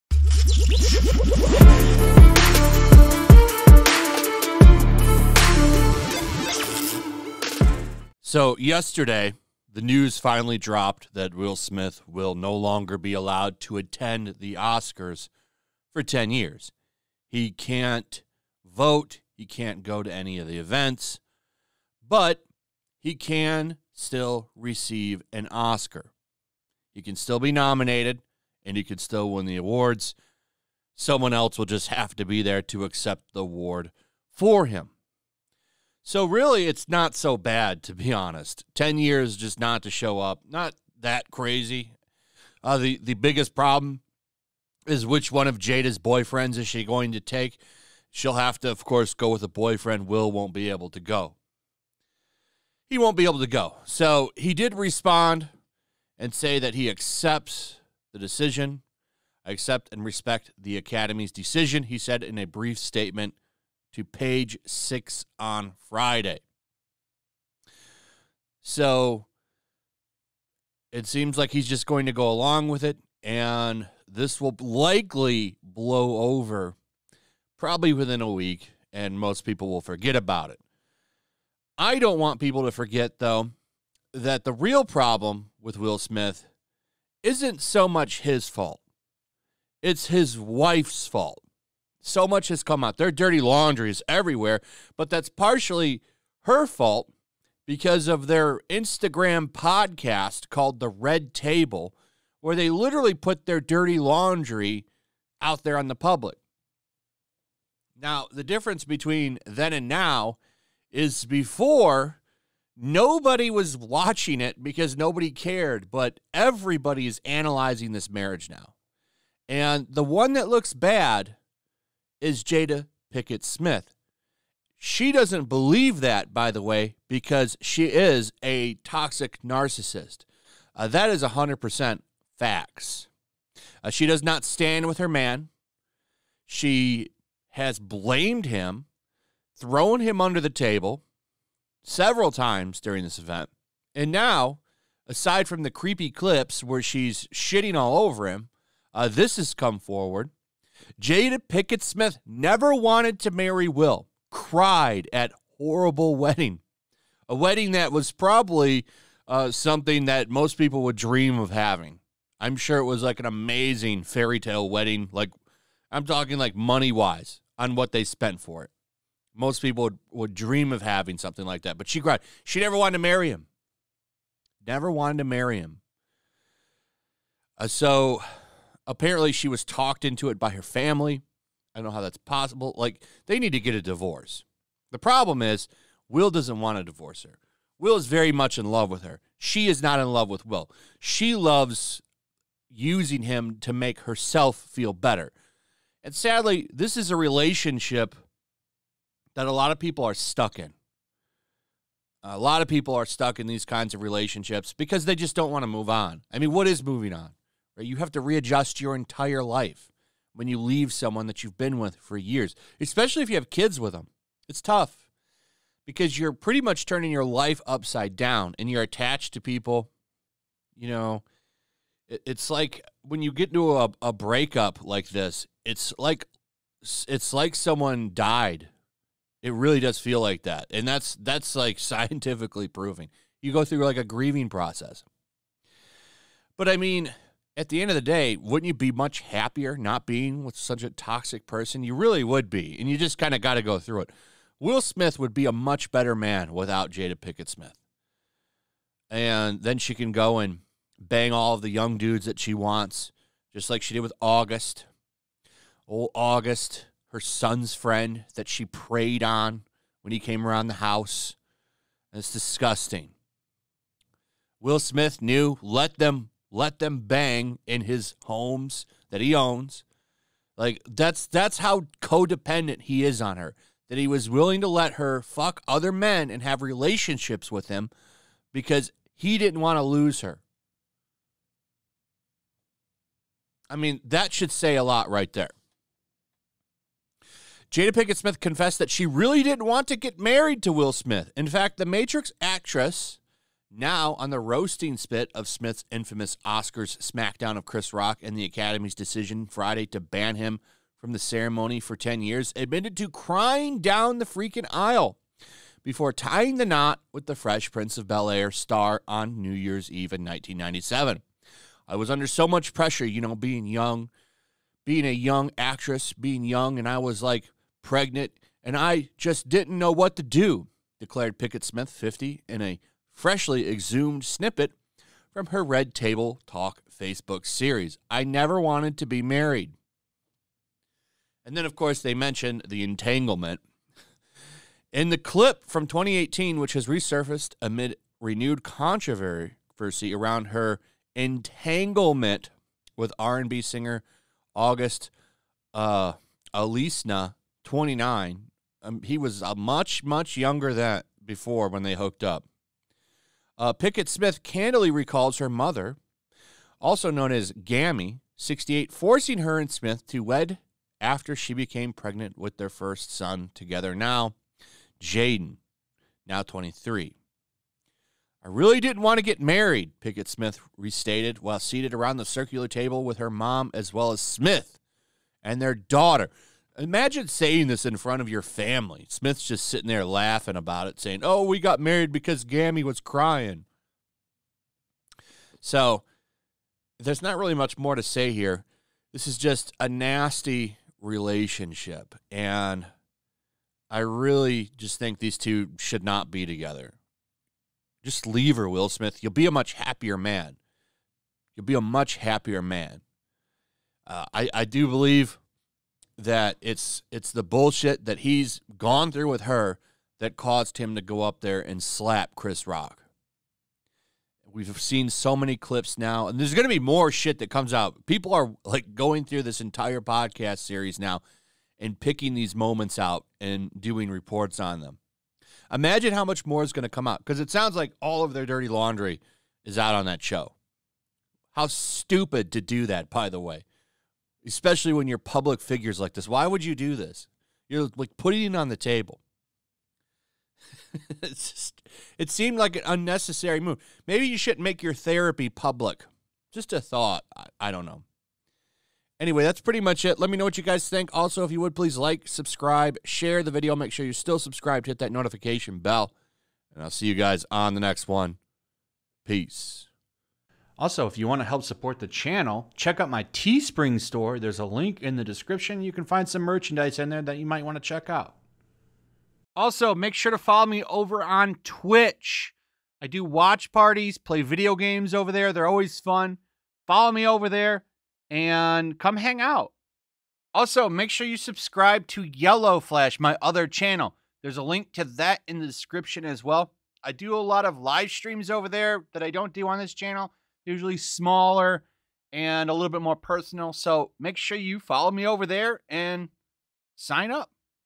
He LIVES under her BOOT!! Jada Pinkett Smith says she NEVER wanted to marry Will Smith!
More information is coming out on the marriage of Jada Pinkett and Will Smith! Looks like it's a lot worse than we thought! Also, Will Smith responds to his Oscars ban!